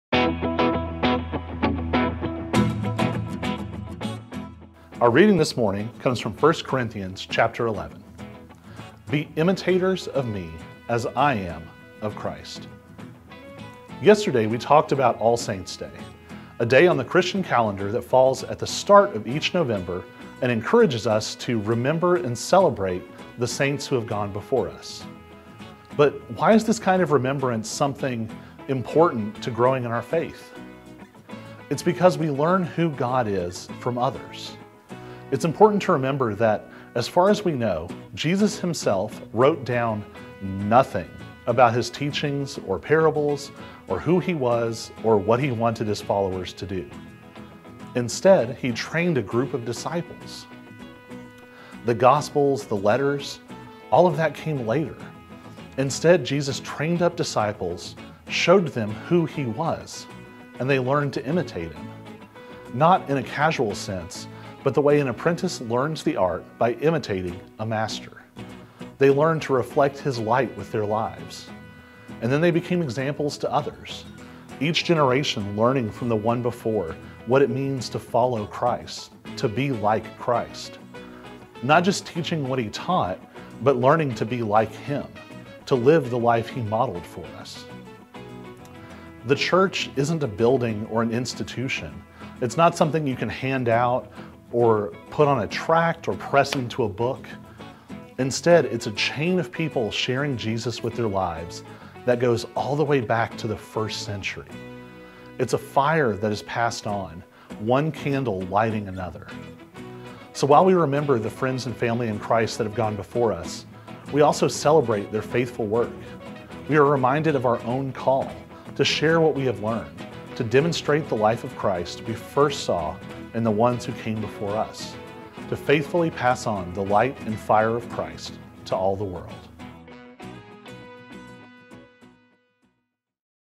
Messages of Hope 103020
Each weekday Colby from Wyatt Park Baptist Church will be bringing you a short message of hope for you to enjoy with your morning cup of Coffee.